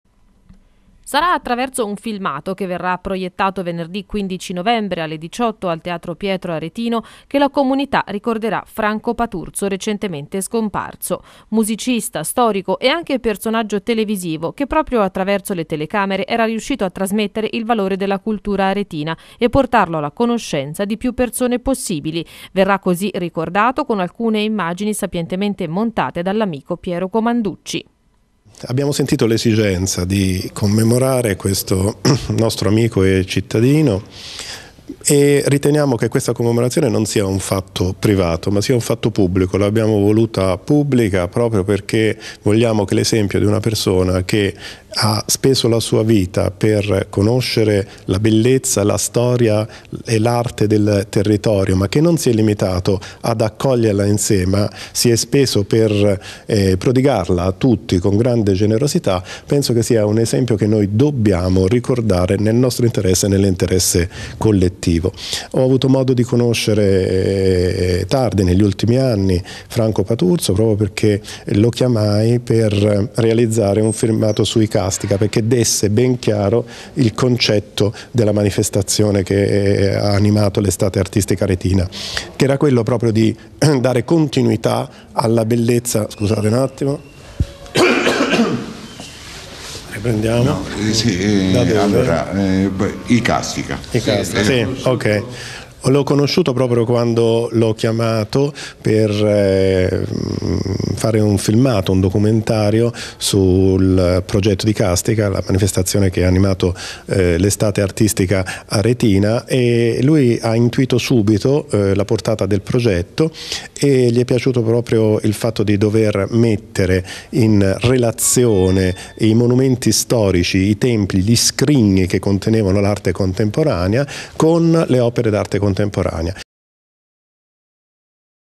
Commemorazione Paturzo
Su iniziativa dell'Amm.ne com.le al teatro Pietro Aretino si svolgerà la cerimonia di commemorazione di Franco Paturzo,